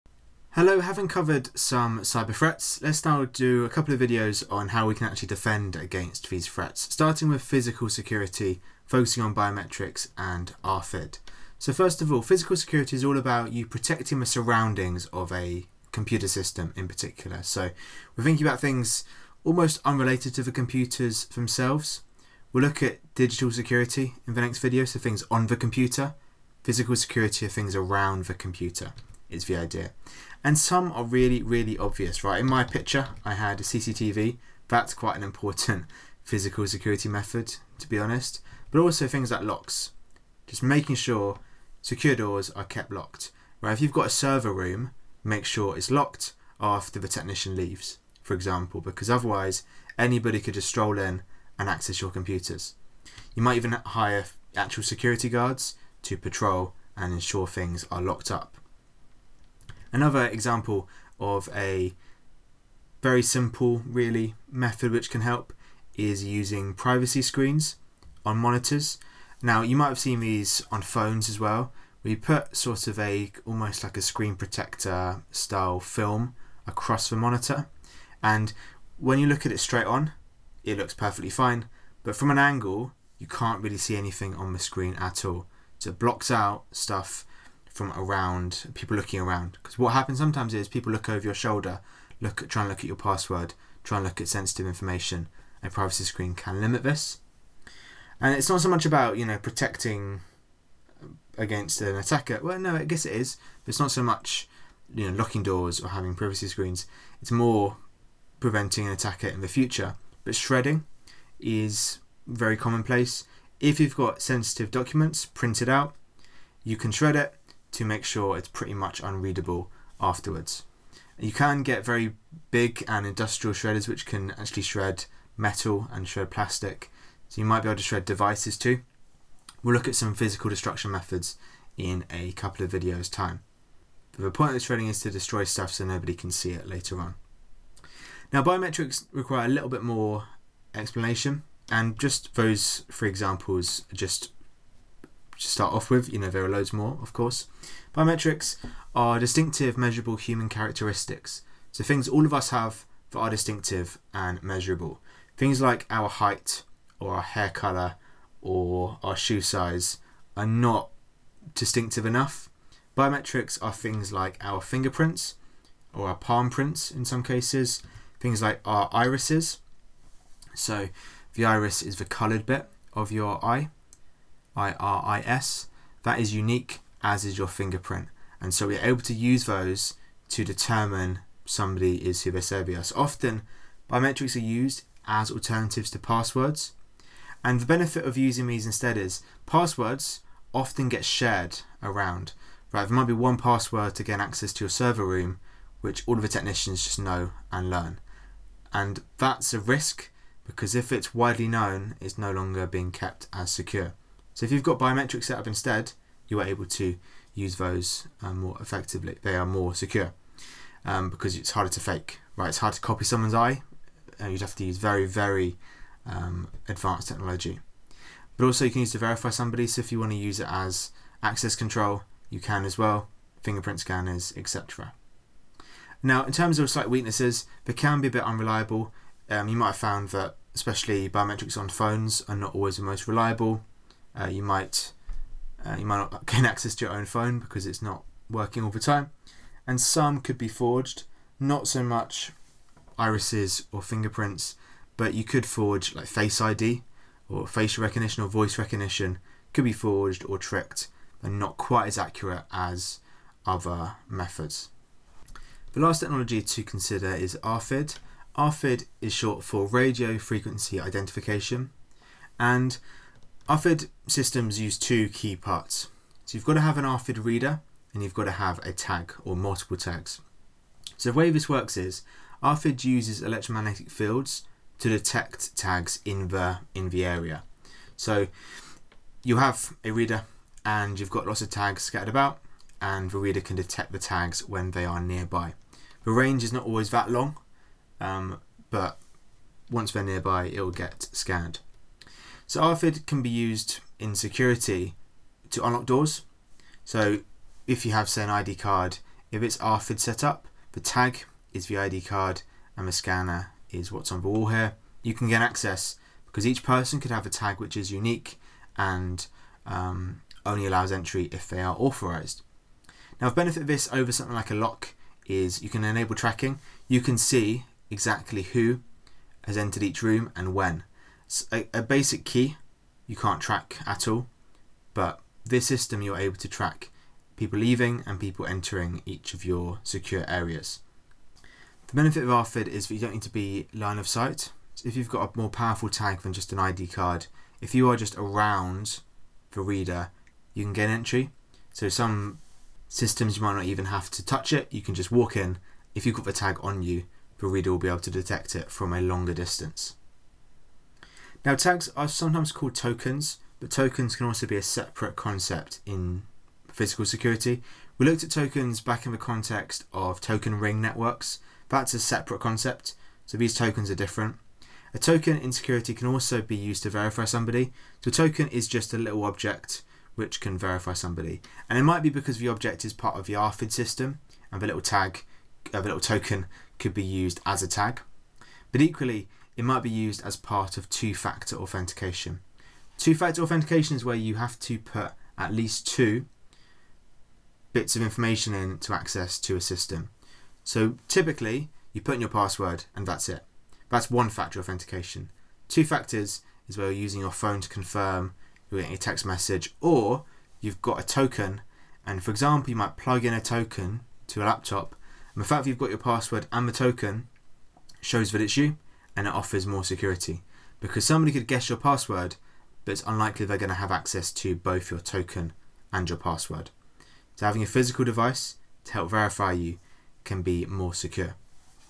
Physical Security Methods
Explaining locks, biometrics, RFID, tokens, privacy screens, and shredding and how they can all be used as part of physical security.

This video is primarily designed for the written Unit 1 exam (Fundamentals of IT) for the OCR Cambridge Technicals (CTEC) Level 3 Information Technology qualifications. Video #4 of LO5 (Understand ethical and operational issues and threats to computer systems).

For the ordered playlist of videos for the CTEC IT Fundamentals of IT (Unit 1) exam,